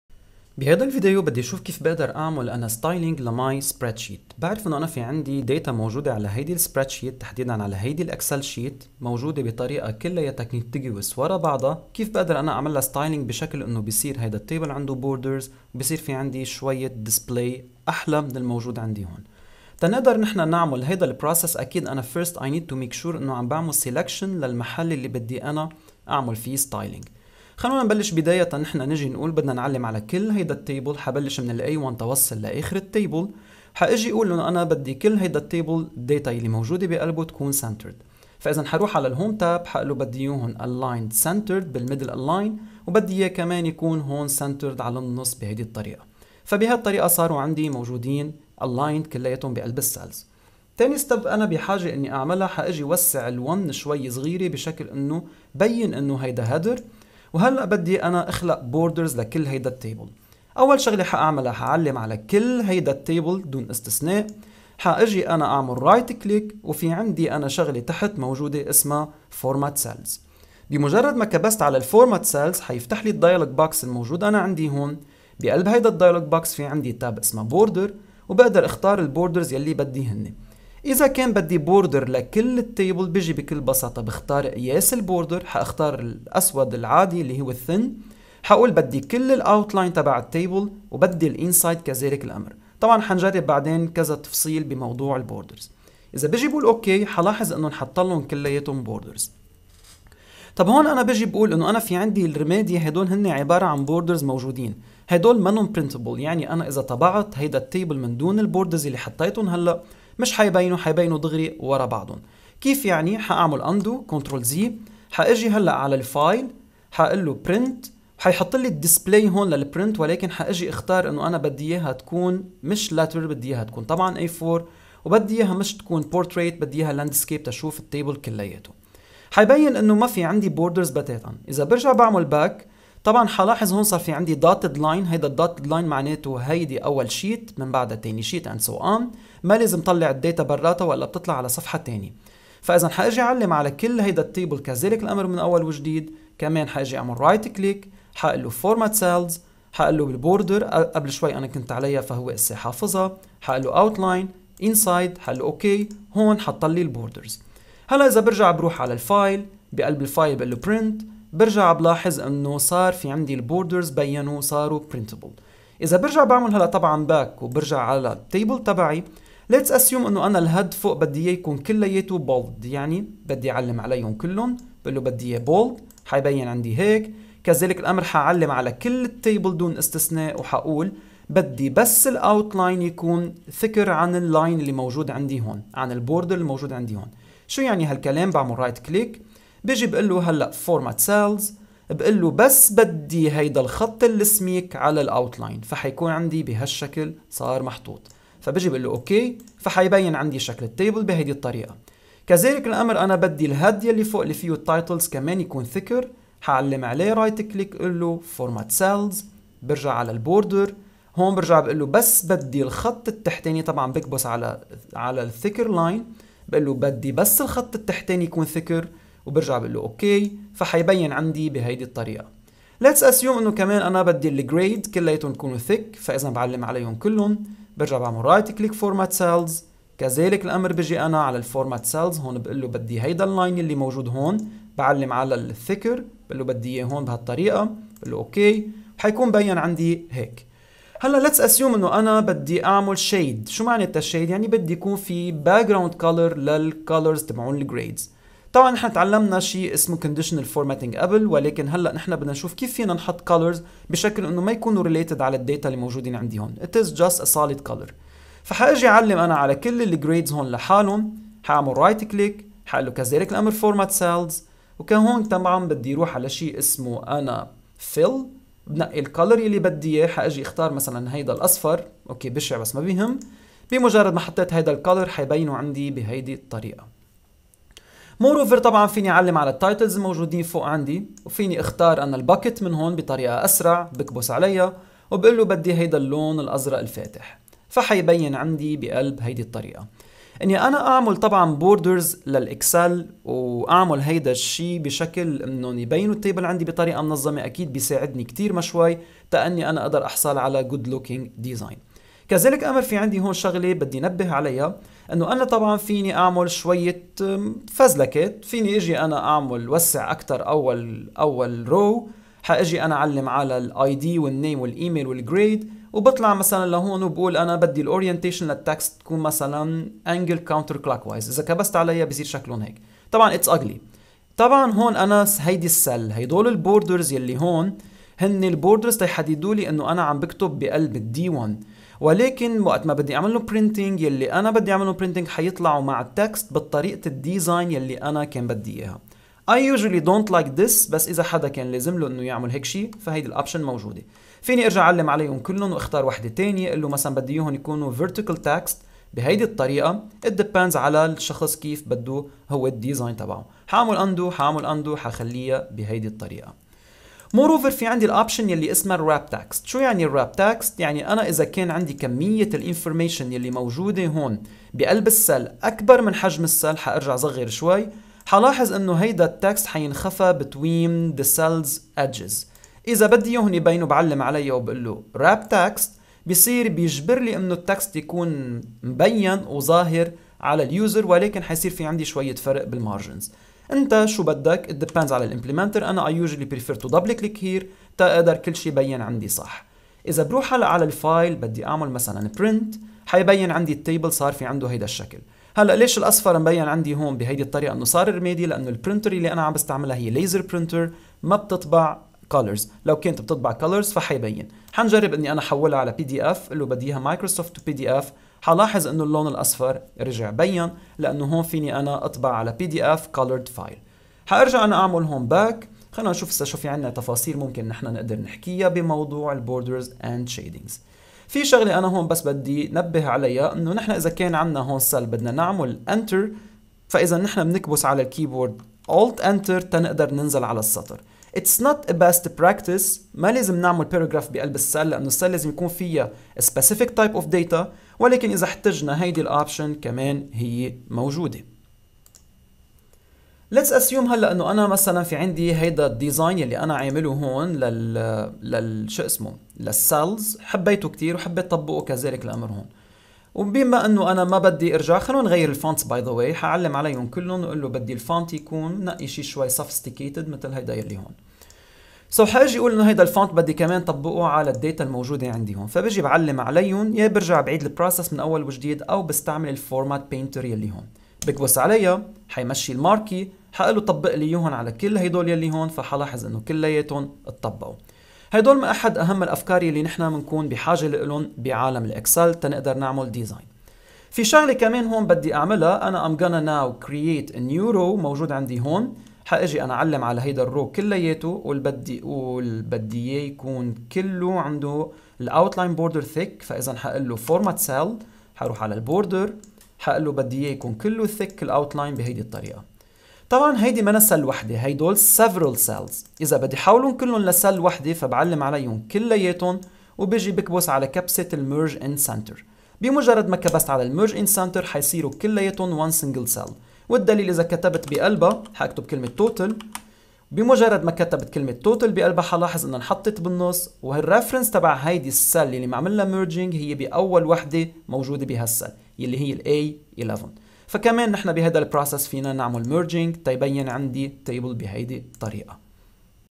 IACS200: 19 - Style your Sheet (Arabic)
This video shows how can we style Excel sheets using the borders and shadings tool.